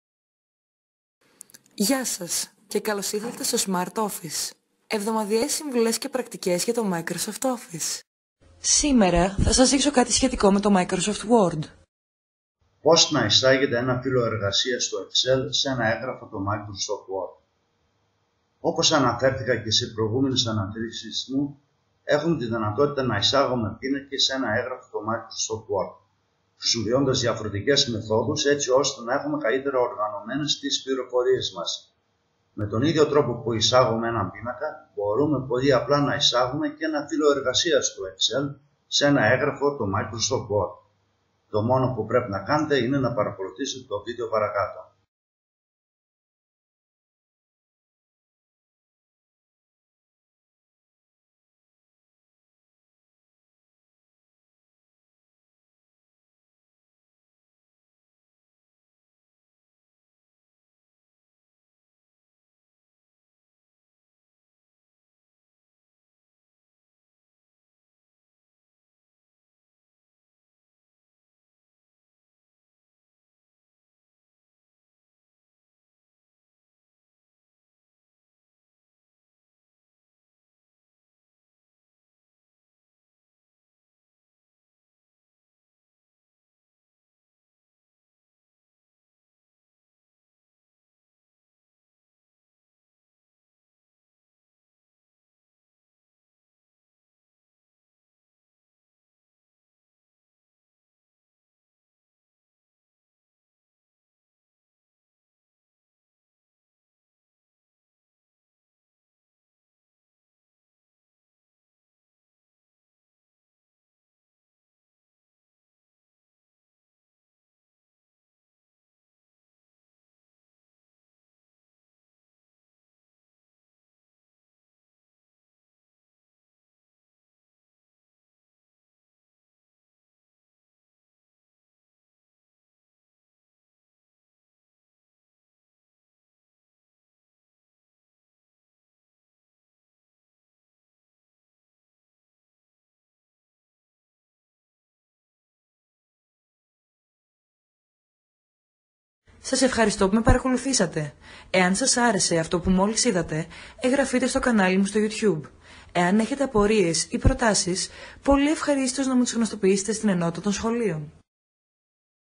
Πώς Να Εισάγεται Ένα Φύλλο Εργασίας του Excel Σε Ένα Έγγραφο Του Microsoft Word;
Όπως αναφέρθηκα και σε προηγούμενες αναρτήσεις μου, έχουμε τη δυνατότητα να εισάγουμε Πίνακες σε ένα Έγγραφο του Microsoft Word, χρησιμοποιώντας διαφορετικές μεθόδους έτσι ώστε να έχουμε καλύτερα οργανωμένες τις πληροφορίες μας.  Με τον ίδιο τρόπο που Εισάγουμε έναν Πίνακα μπορούμε πολύ απλά να Εισάγουμε και ένα Φύλλο Εργασίας του Excel σε ένα Έγγραφο του Microsoft Word.
Πρώτα, σε περίπτωση που κάποιοι δεν γνωρίζουν ένα Φύλλο Εργασίας αποτελείται από Γραμμές και Στήλες που μπορείτε είτε να ταξινομήσετε ή να φιλτράρετε Δεδομένα και επίσης να υπολογίσετε Αριθμητικά Δεδομένα.  Μπορείτε επίσης να υπολογίσετε δεδομένα χρησιμοποιώντας μαθηματικές Συναρτήσεις  με τα Δεδομένα των Κελιών.

Εάν ενδιαφέρεστε να μάθετε πώς, απλώς παρακολουθήστε το παρακάτω βίντεο.
Εάν ενδιαφέρεστε να μάθετε πώς γίνεται βήμα, βήμα μπορείτε να διαβάσετε την πλήρη ανάρτηση στον παρακάτω σύνδεσμο του ιστολογίου μου: